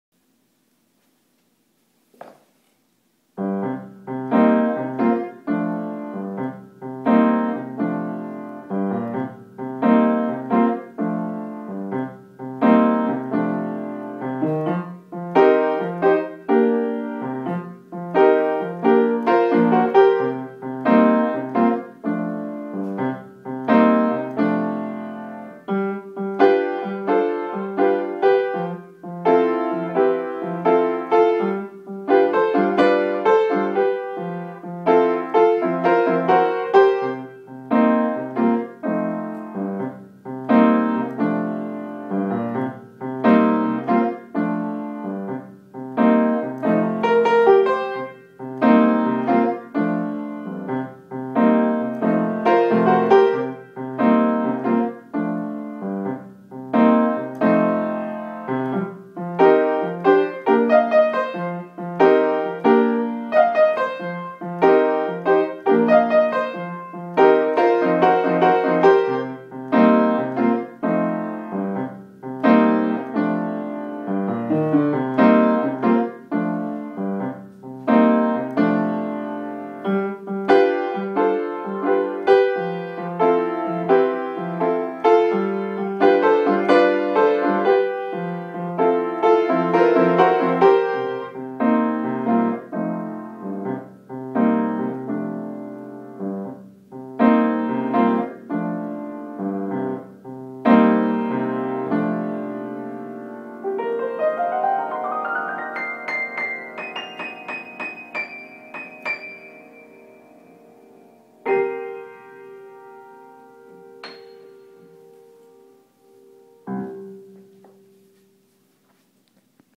Jay Shields on Piano
Improv on a I IV V Blues Progression.